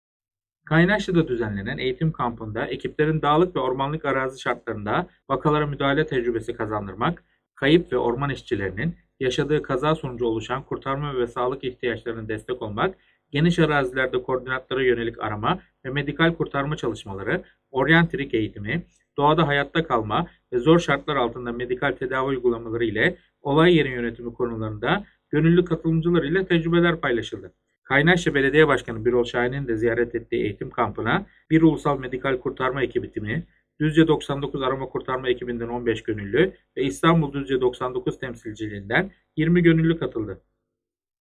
Düzce UMKE Eğitim Kampına Katıldı
Düzce Ulusal Medikal Kurtarma Ekibi (UMKE) olarak Duzce99 arama kurtarma ekibi ve İstanbul Düzce99 temsilciliğince Kaynaşlı Yeni Yurt yaylasında ortaklaşa düzenlenen eğitim kampına katıldı.